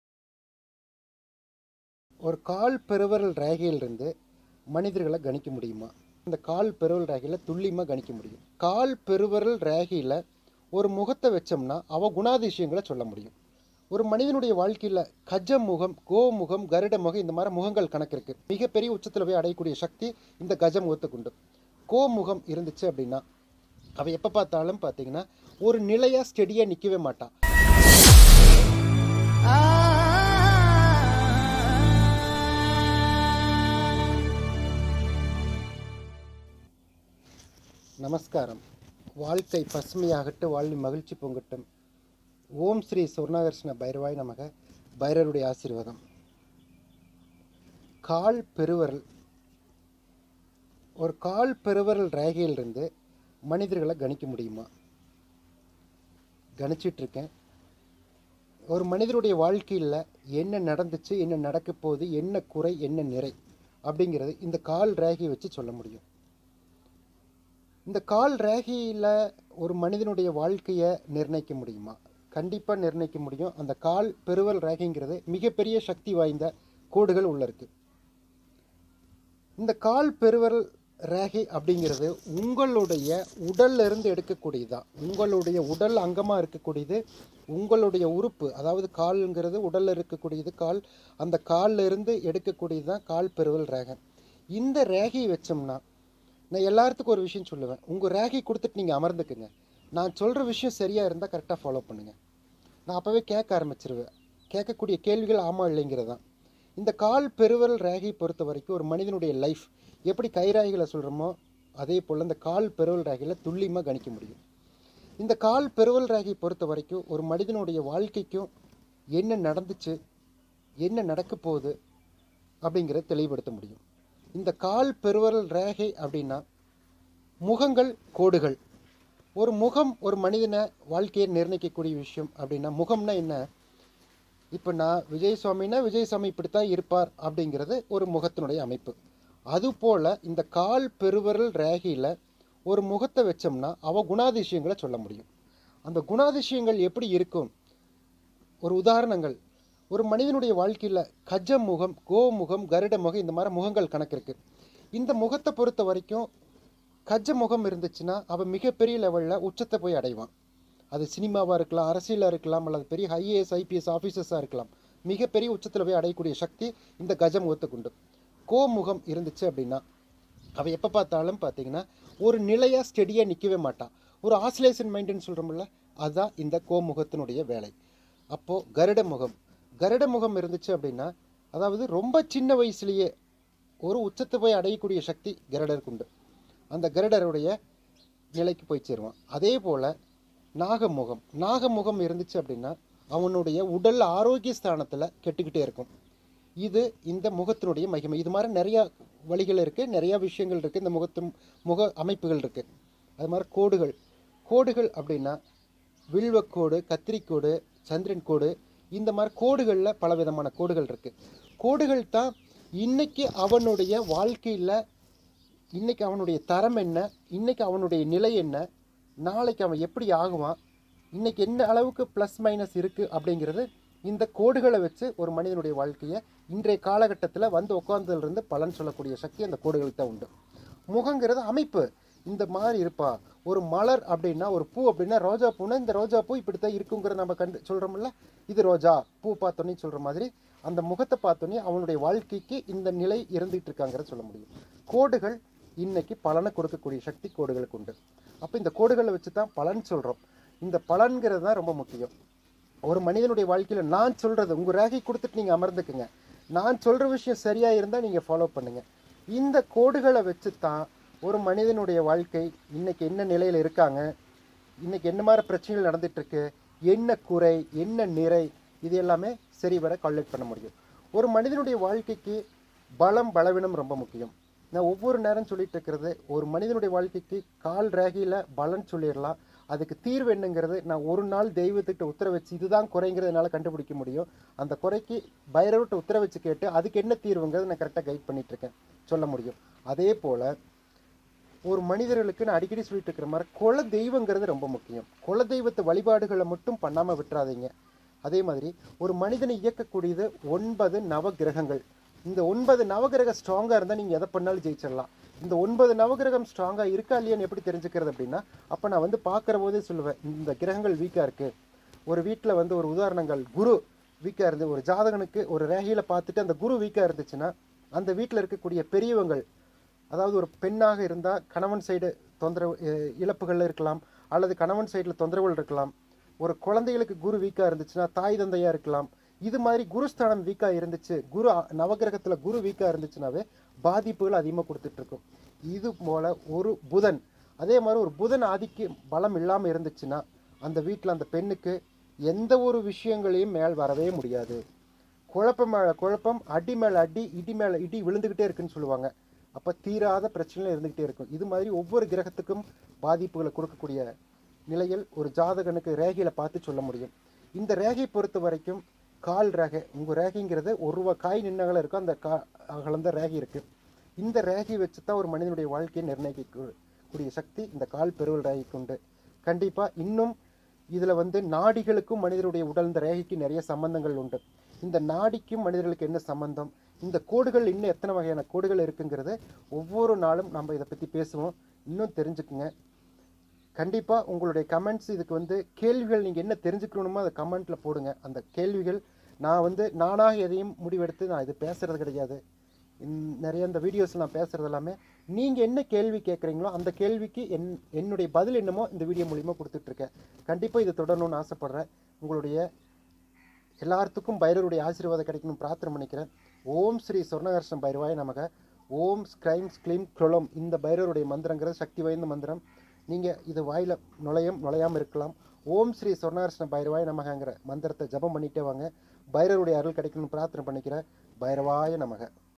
கால் பெருவிரல் ரேகையில் மனிதனின் வாழ்க்கை
கால் பெருவிரல் ரேகையில் மனிதனின் வாழ்க்கையை எப்படி துல்லியமாக கணிப்பது?

கால் ரேகையில் உள்ள கோடுகளின் பலன் என்ன?

கஜ முகம், கோ முகம், கருட முகம் என்றால் என்ன?

நம் இன்றைய நிலை என்ன, நம் எதிர் காலத்தை பிரகாசமாக மாற்றுவது எப்படி?

விஜய் சுவாமிஜி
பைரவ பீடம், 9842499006